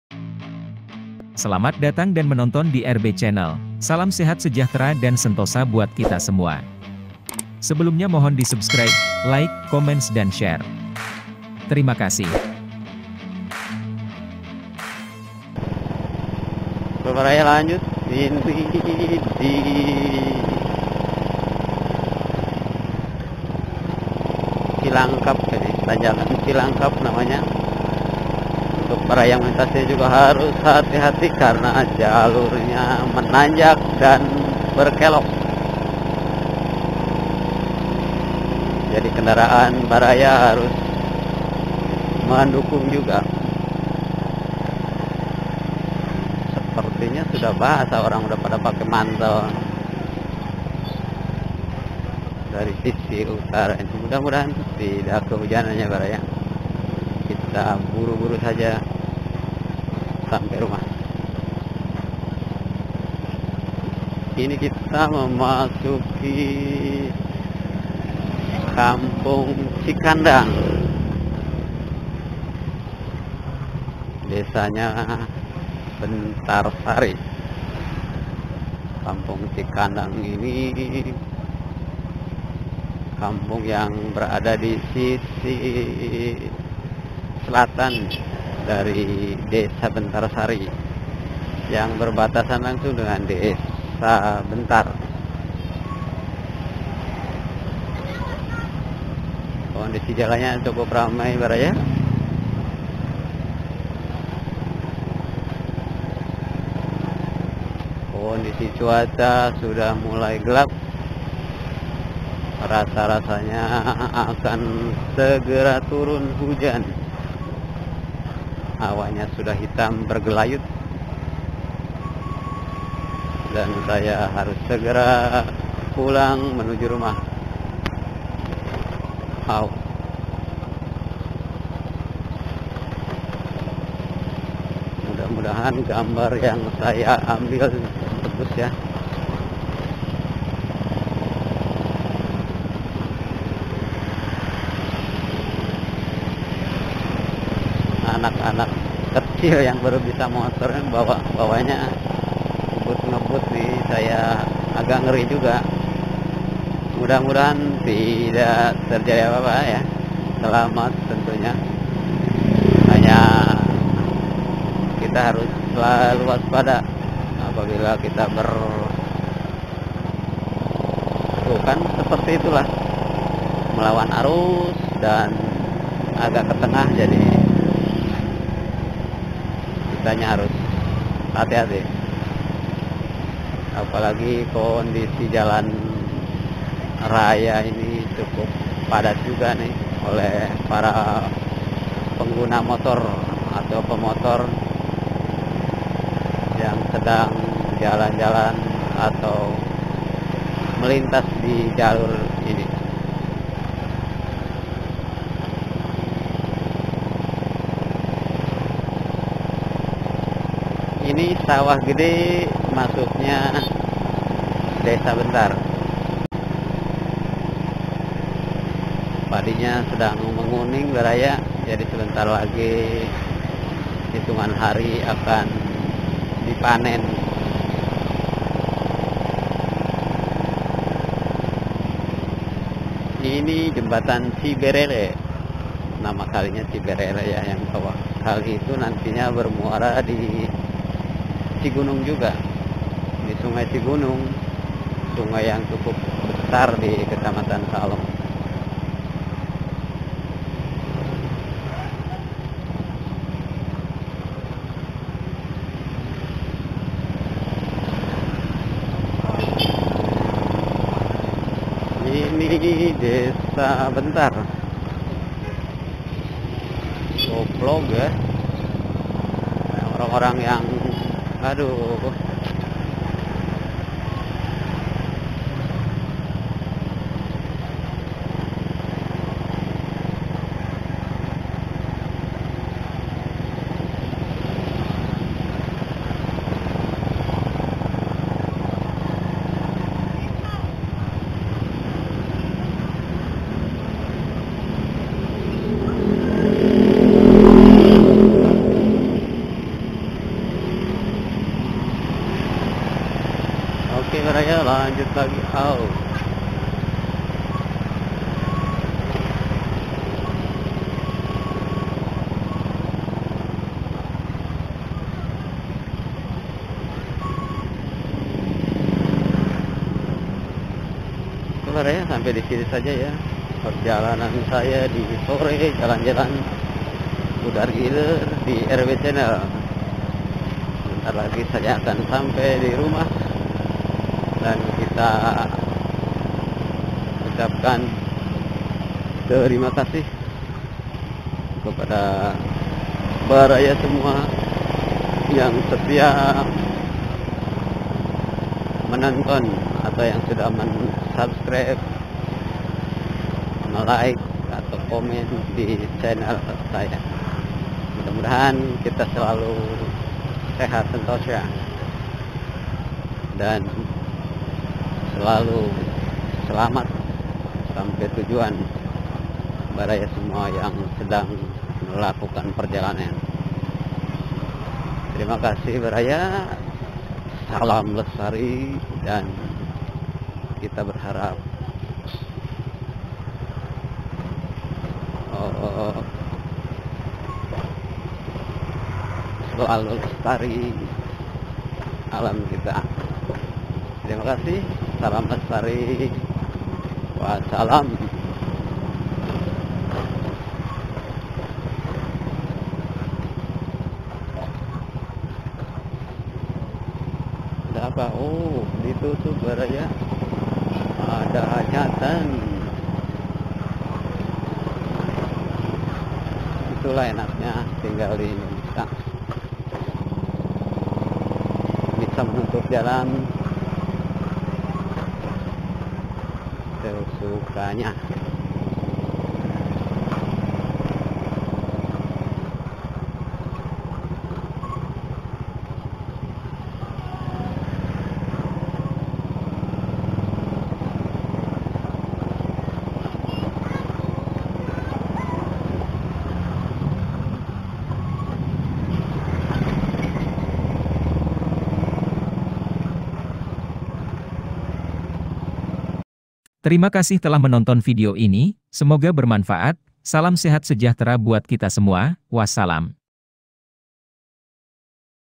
CIGARENG - BENTARSARI
Melintas jalur utama Cigareng - Bentarsari